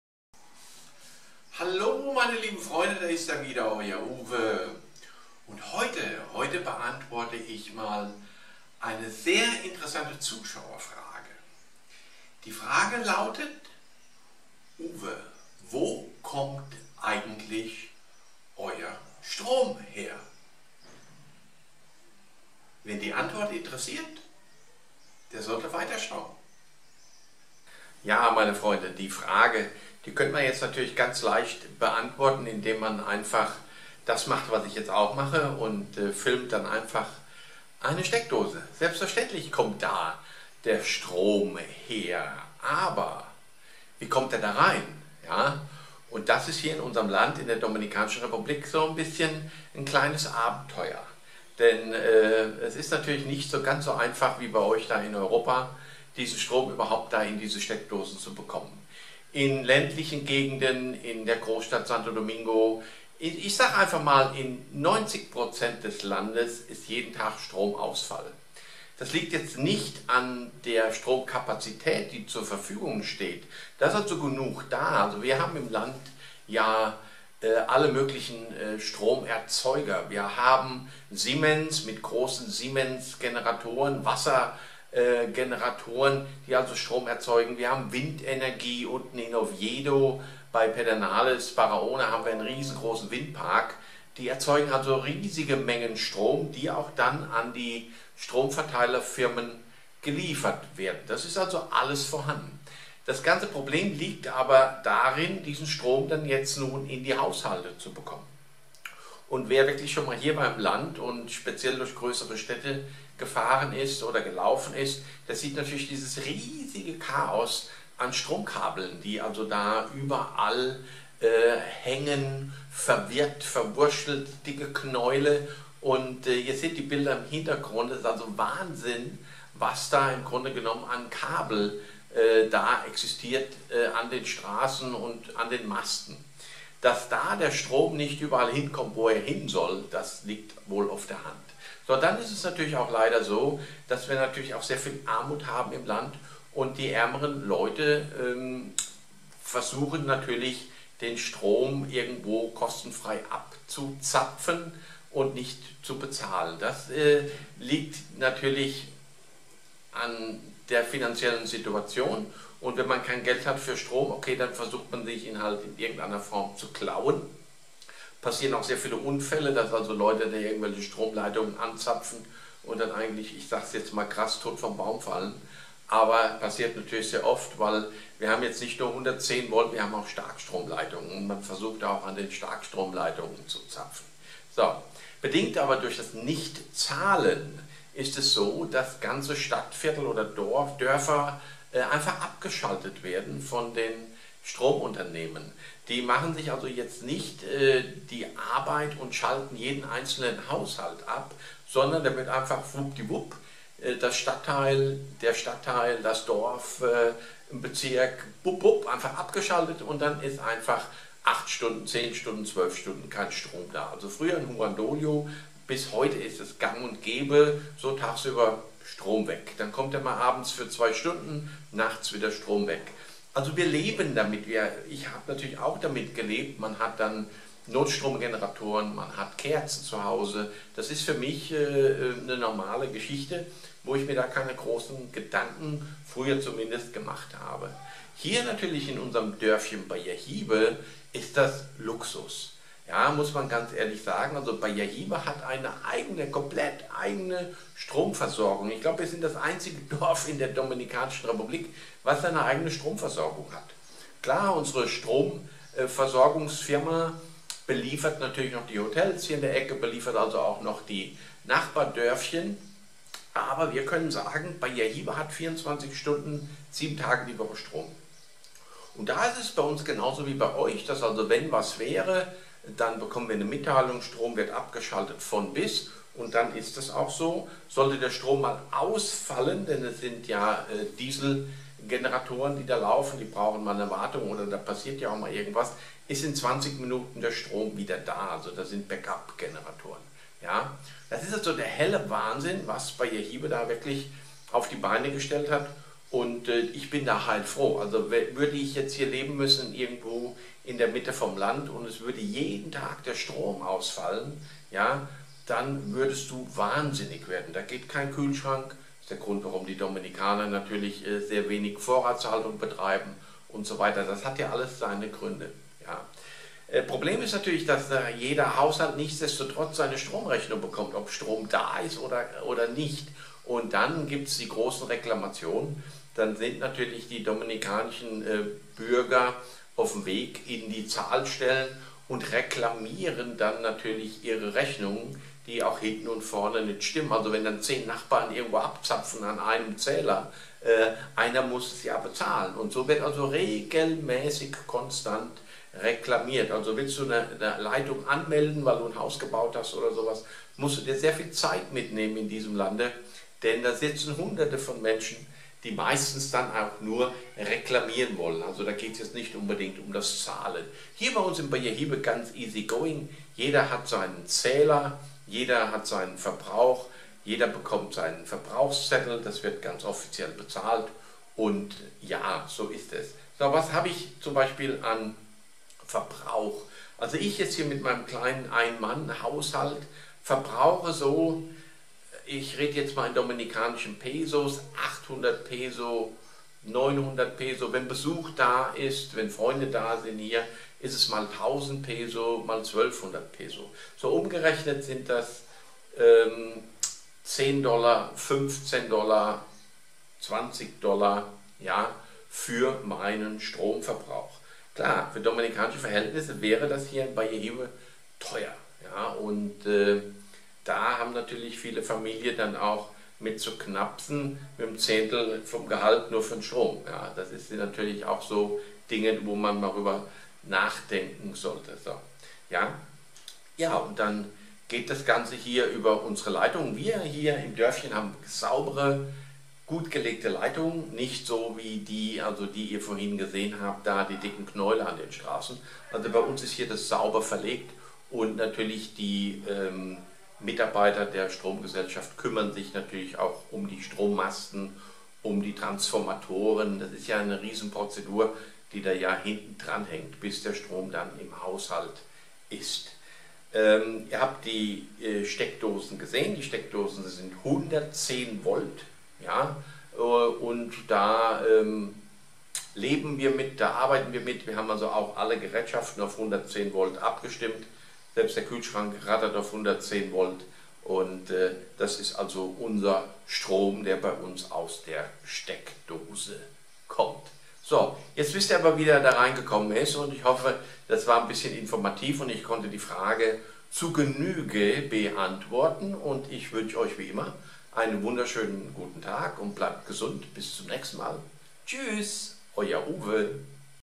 Strom in der Dominikanischen Republik
Wie kommt der Strom in unsere Steckdosen, Probleme mit Stromkabeln. Rechnungstellung und Stromdiebstahl. Bayahibe hat eigene Stromversorgung.